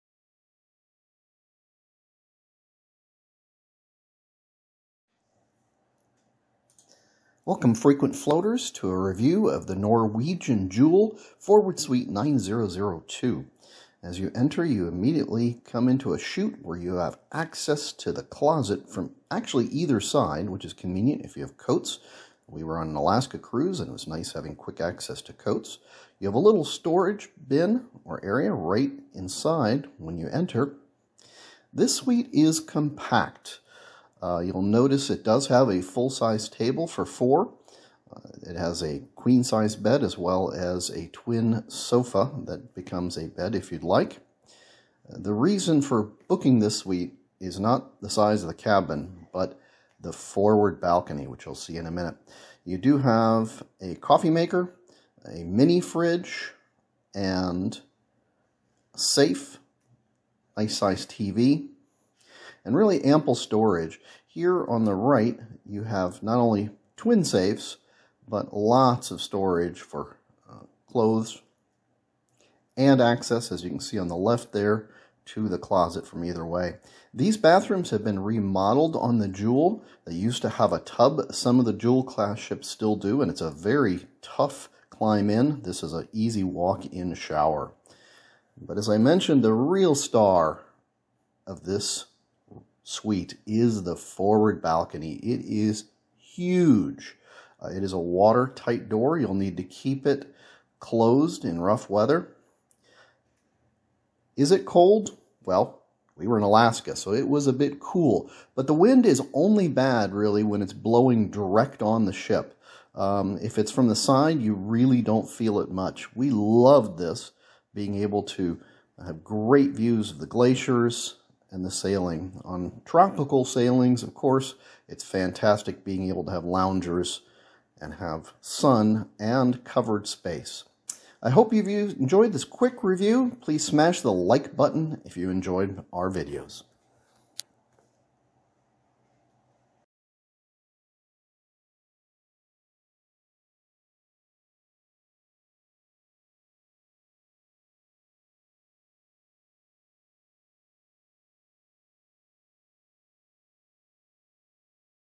Norwegian Jewel Suite 9002 Forward Review Large Balcony
A quick review of the Norwegian Jewel Suite 9002 Forward Large Balcony